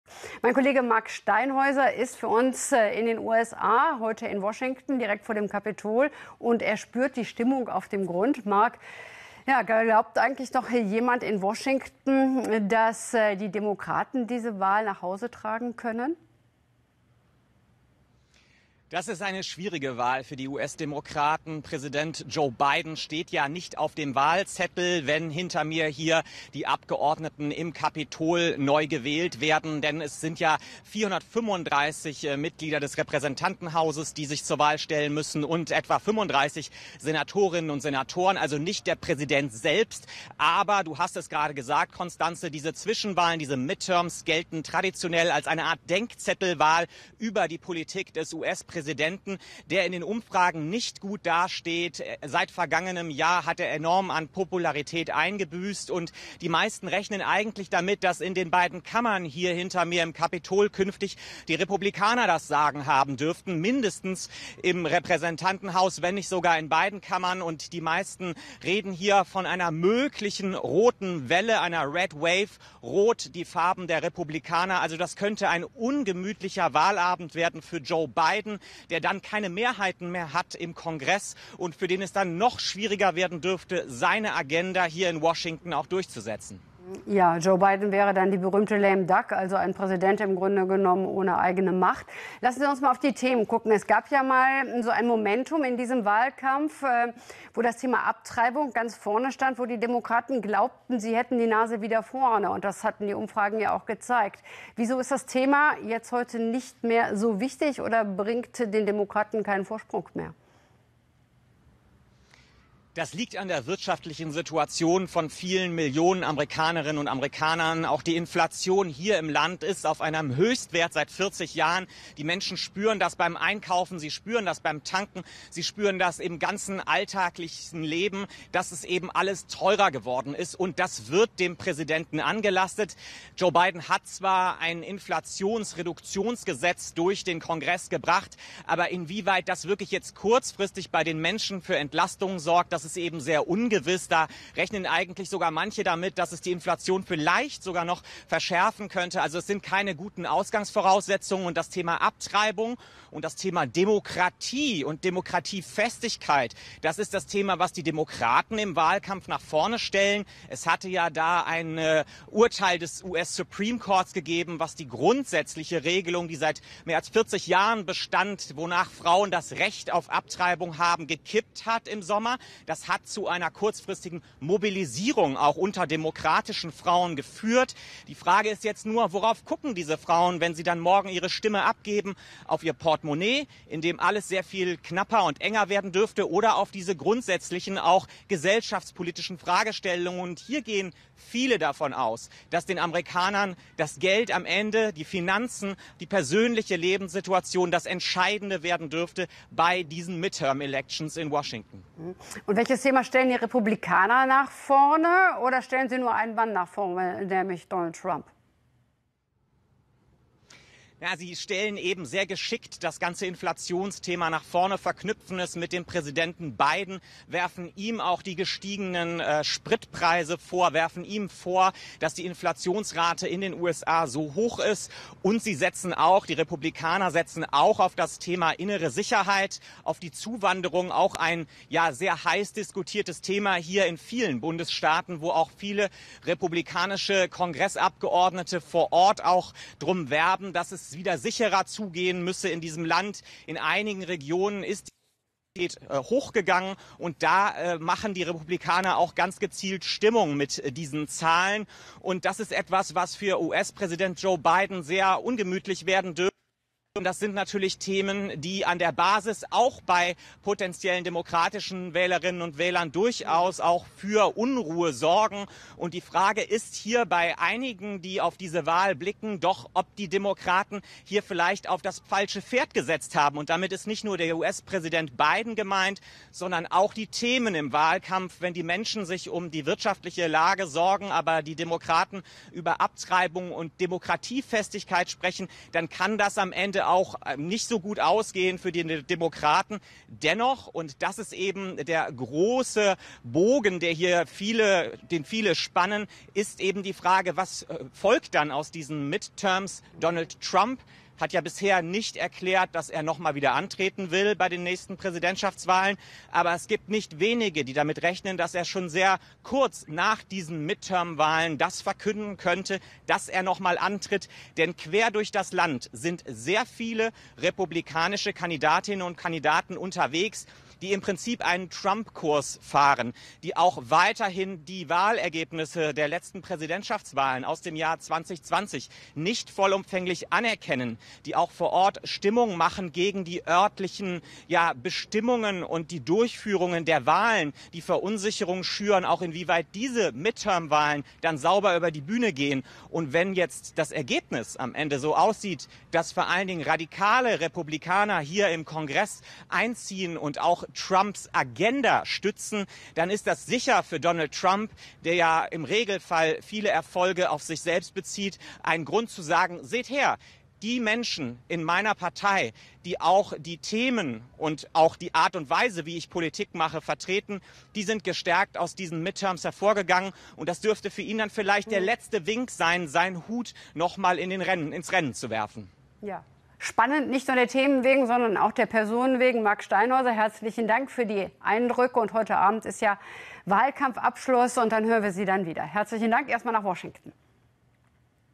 Midterms: Analyse von Marc Steinhäuser zu den US-Wahlen
Wer macht das Rennen bei den Midterm-Elections in den USA? Eine Analyse von phoenix-Reporter Marc Steinhäuser.